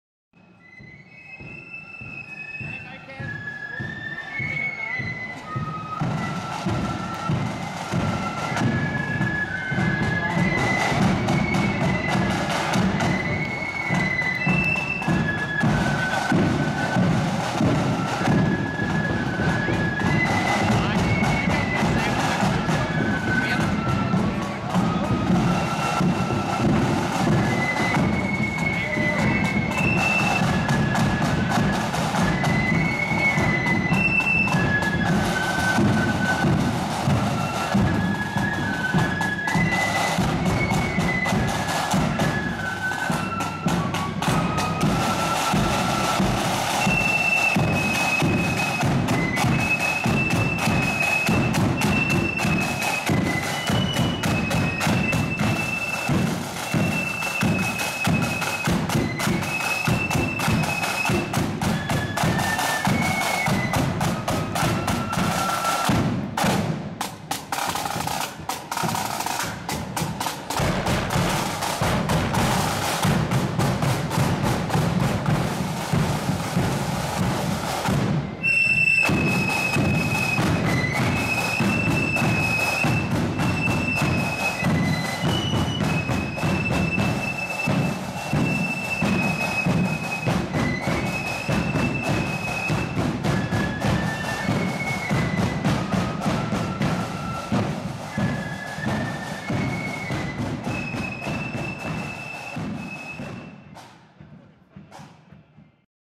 Armagh True Blues @ Pride of Knockmore Festival 2019
Armagh True Blues at Pride of Knockmore annual Festival of Marching Bands in Lisburn City Friday 10th May 2019